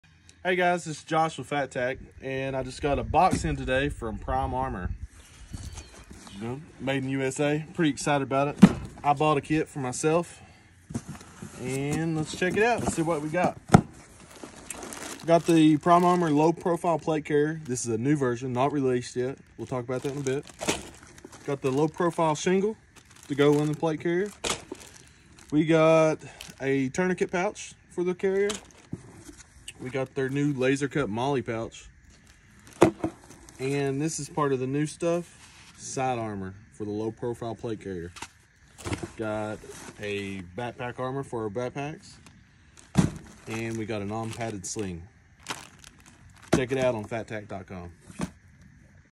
A low profile plate carrier with side armor!!!!!!
Join me in this exhilarating unboxing video as I dive into a package I received from Prime Armor! Bursting with excitement, I can’t wait to reveal the latest innovations in body armor that I purchased with my hard-earned money. From cutting-edge protective gear to essential accessories, this unboxing is packed with surprises! Don’t miss out on your chance to gear up with the best in the industry. Check out fattac.com to get your hands on the same top-notch equipment featured in this video. Let’s dive in and discover what Prime Armor has in store for us!